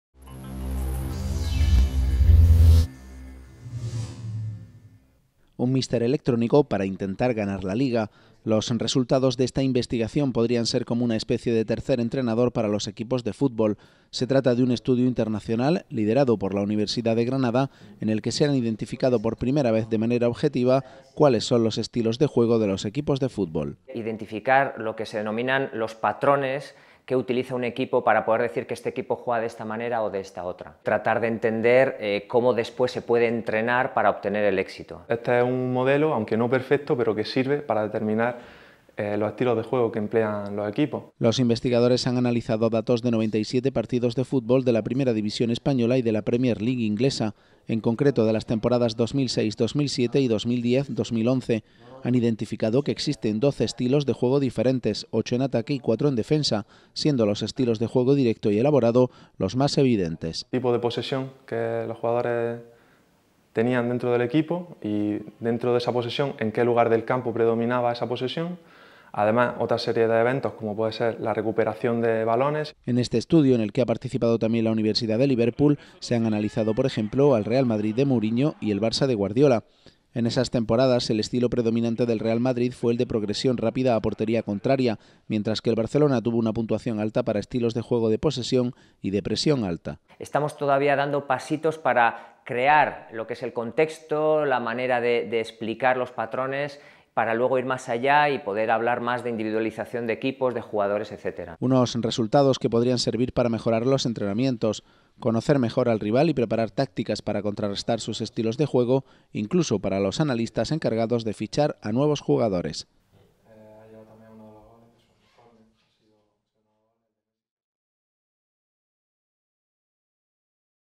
HDL Un entrenador electrónico para ayudar a ganar a los equipos de fútbol
Científicos identifican por primera vez de manera objetiva cuáles son los estilos de juego de los equipos de fútbol. Se trata de un estudio internacional, liderado por la Universidad de Granada, que ha analizado 97 partidos de las ligas española y la inglesa. Sus conclusiones podrían ayudar a mejorar entrenamientos y a preparar partidos.